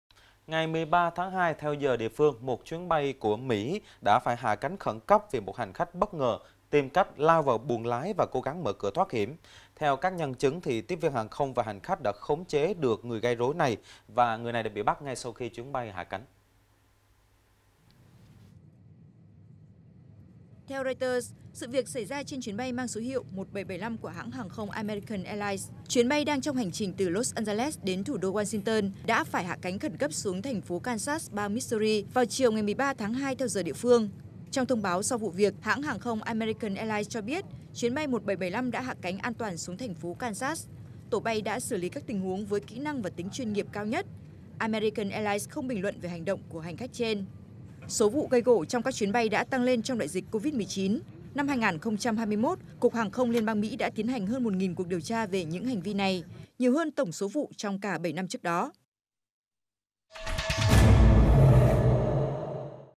MỸ: MÁY BAY HẠ CÁNH KHẨN CẤP VÌ HÀNH KHÁCH GÂY RỐI | VTC9
VTC9 | MỸ: MÁY BAY HẠ CÁNH KHẨN CẤP VÌ HÀNH KHÁCH GÂY RỐI